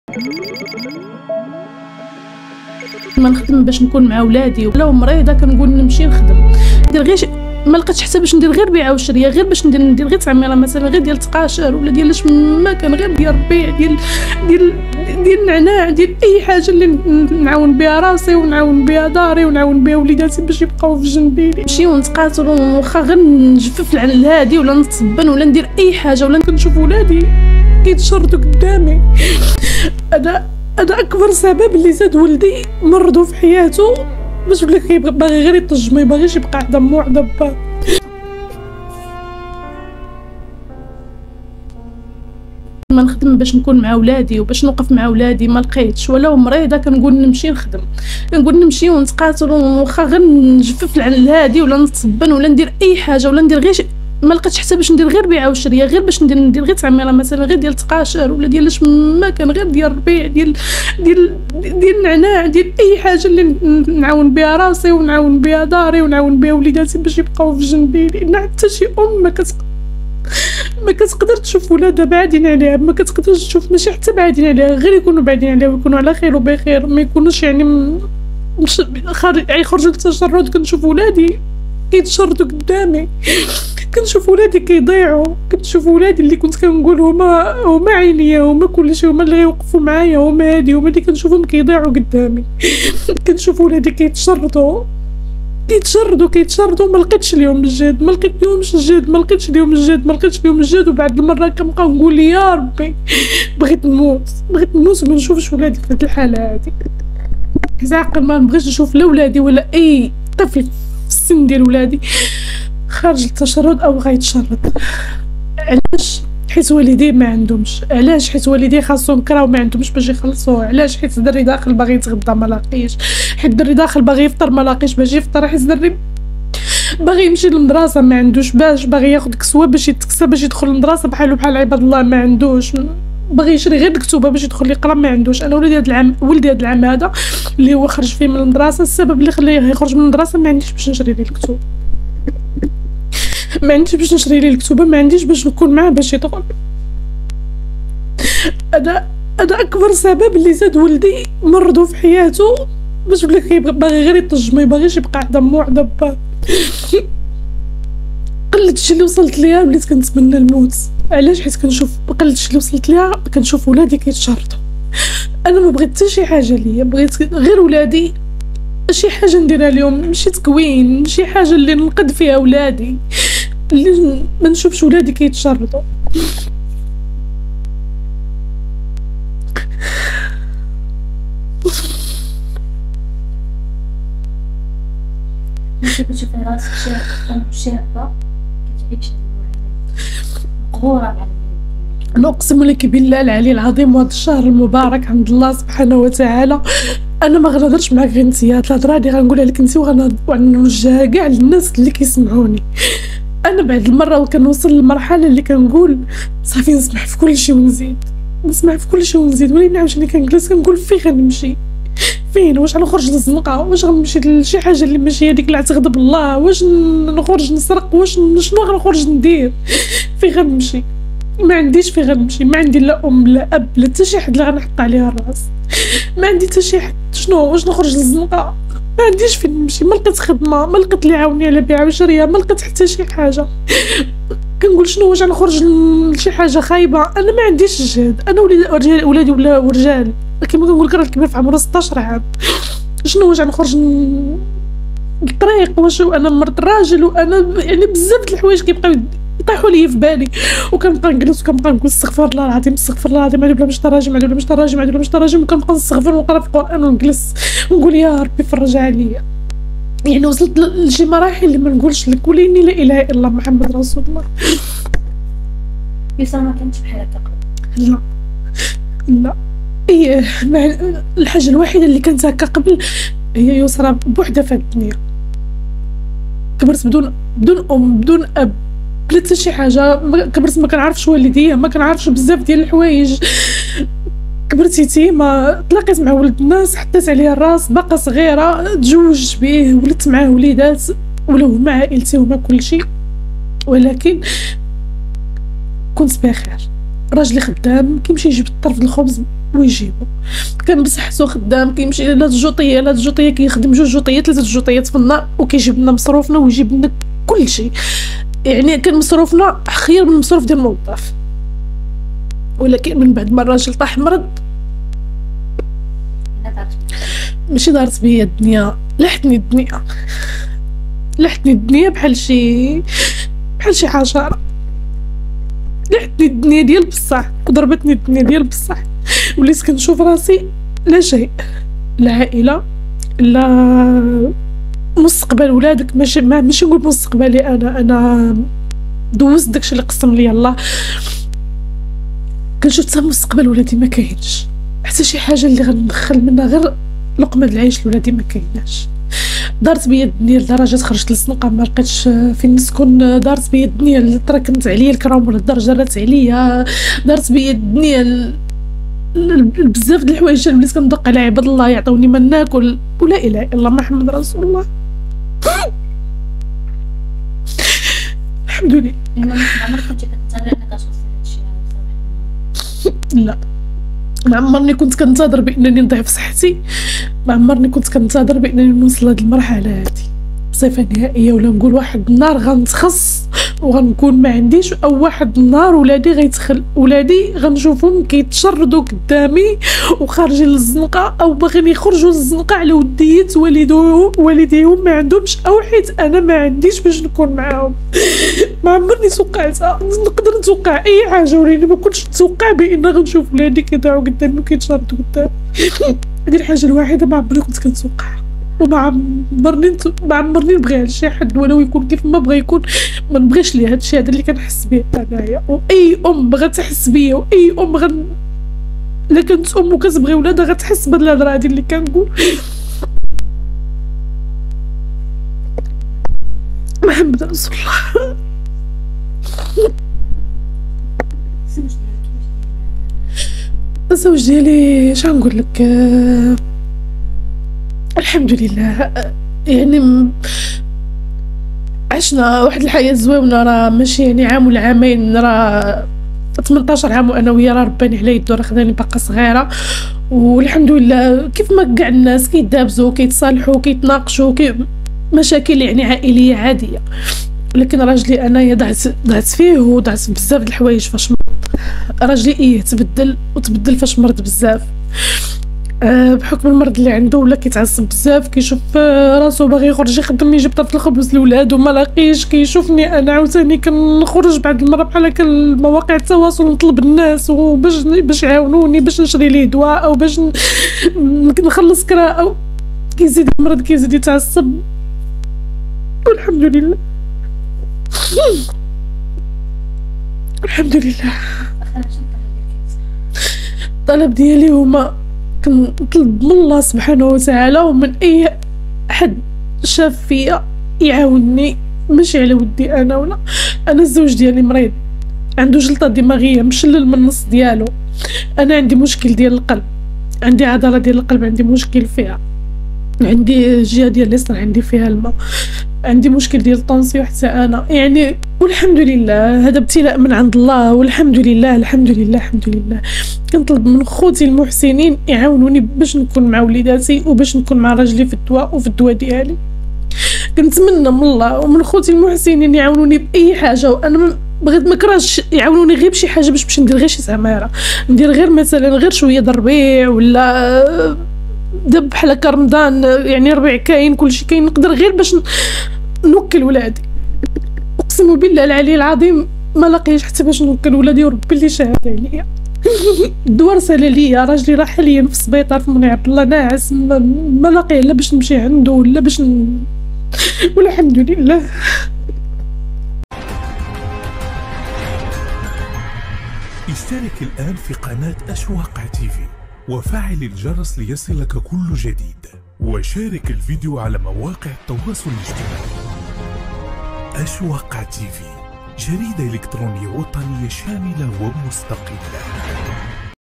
بالدموع من بعد ما راجلها مرض دارت بيها دنيا: بغيت ندير غير شي بيعة وشرية عمرني توقعت غادي نوصل لهاد
بالدموع من بعد ما راجلها مرض دارت بيها دنيا: بغيت ندير غير شي بيعة وشرية عمرني توقعت غادي نوصل لهاد المرحلة